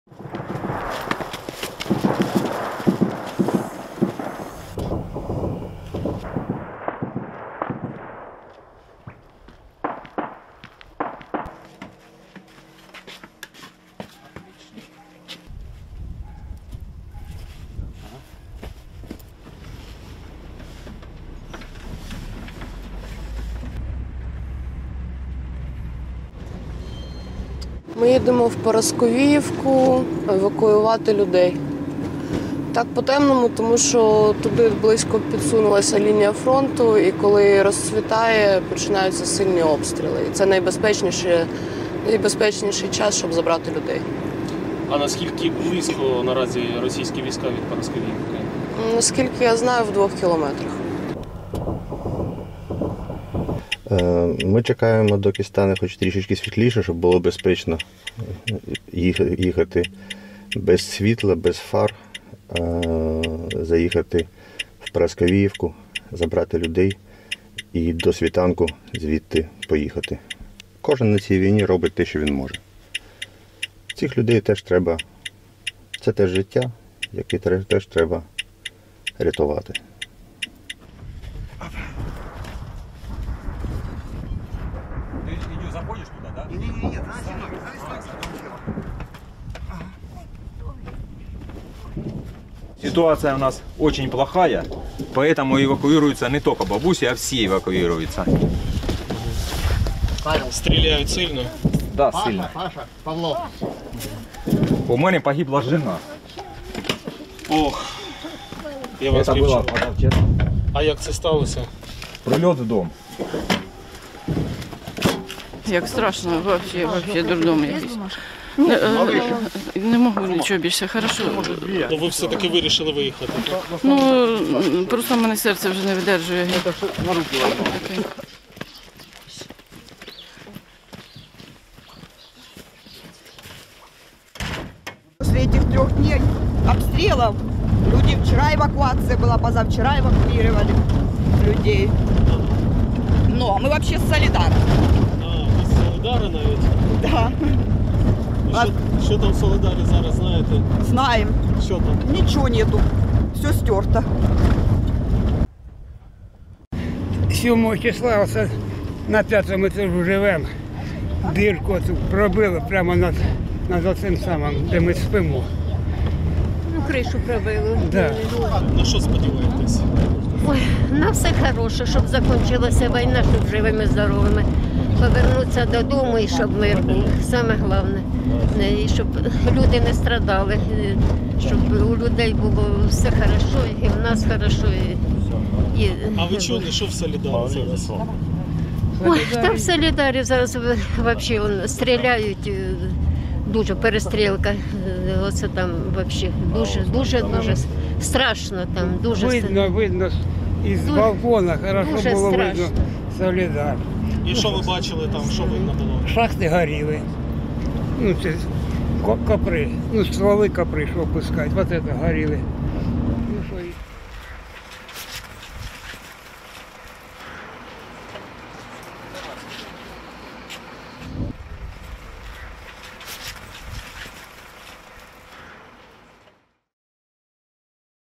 Volunteers Evacuate Ukrainian Civilians From Villages Near Soledar
Seven Ukrainian civilians, including a woman with reduced mobility and a 5-year-old girl, were evacuated by volunteers on January 13 from Paraskoviyivka, a village located about 5 kilometers from Soledar, where fierce fighting is taking place.

Check (and bookmark) our regularly updated briefing on the latest situation and outstanding reporting